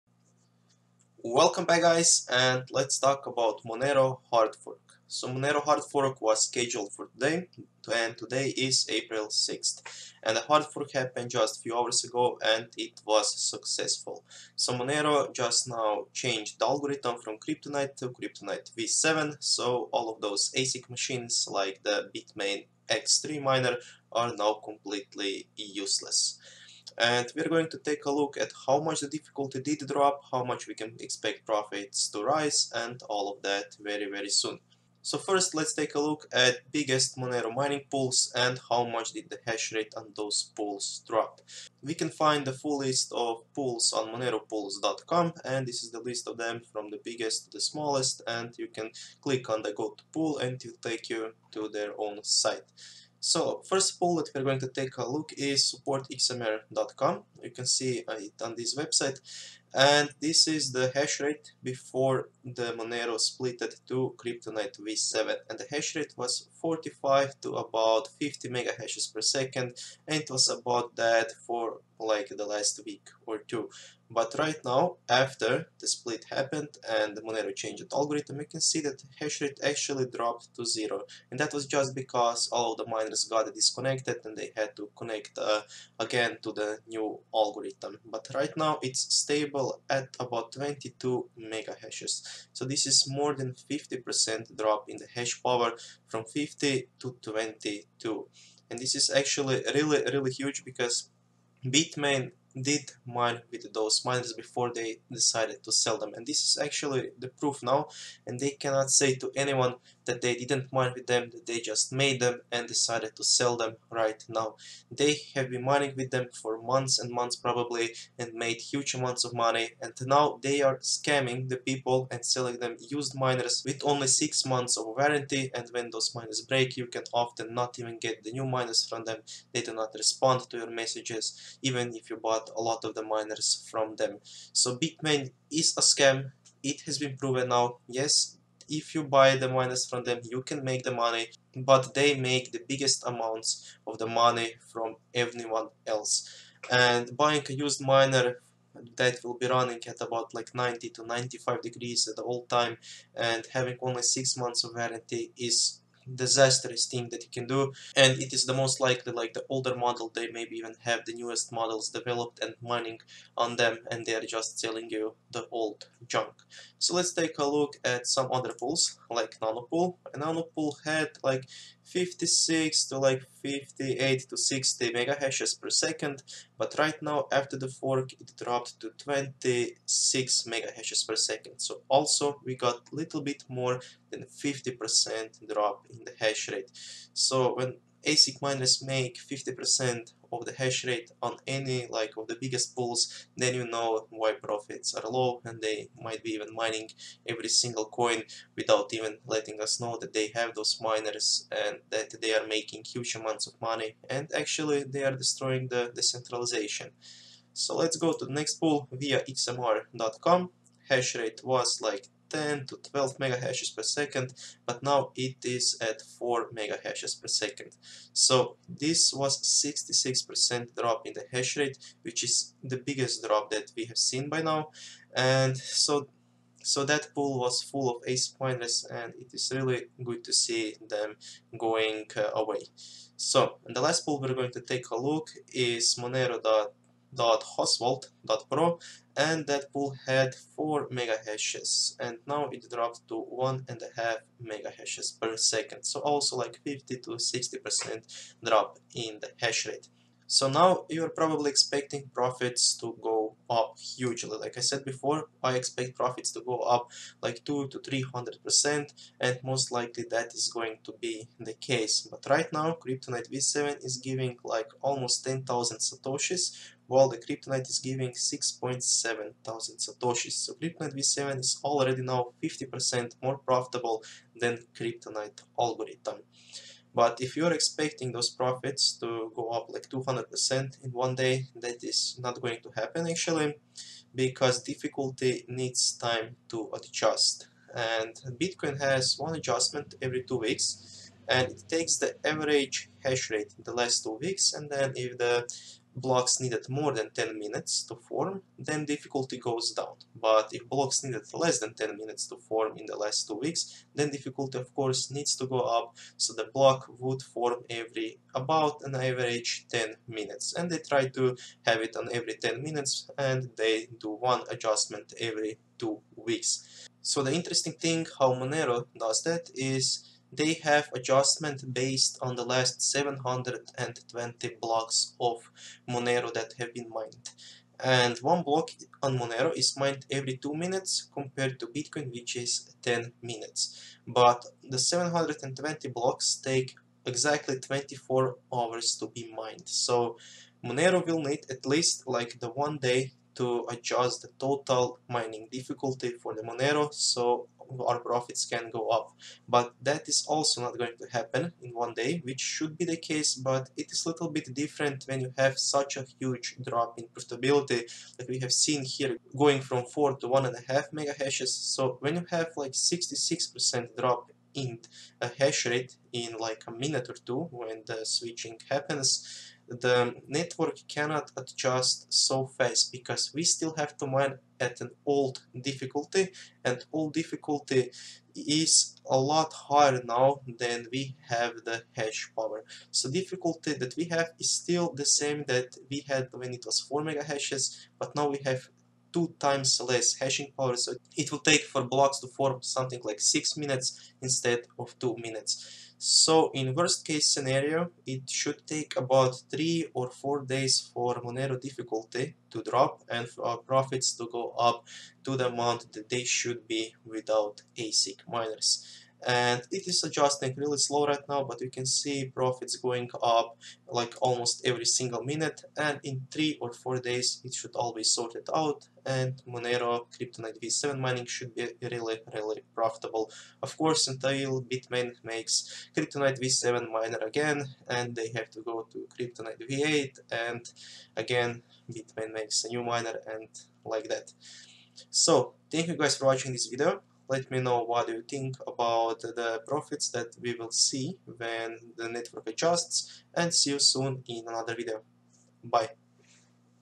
Monero hard fork and CryptonightV7 mining profits
Timeline of Future Antminer X3-related events

Cryptonight-based coin IPBC will begin ASIC resistance tests.

Monero hard-fork will take place, Cryptonight ASICs will likely no longer be able to mine Monero efficiently.
Monero hashrate and difficulty will likely drop significantly, potentially allowing CPU/GPU Monero mining to be much more profitable.

Cryptonight-based coin GRAFT will become ASIC resistant.

MoneroV (XMV) hard-fork will take place. This is unrelated to the April 6th hard-fork, and many people have claimed this is a scam/has security risks. We cover this in an earlier guide.

May 2018

Cryptonight-based coin LeviarCoin will be "switching to Monero's code" (suggesting it will become ASIC resistant)

Unspecified date

Cryptonight-based Intense Coin will become ASIC resistant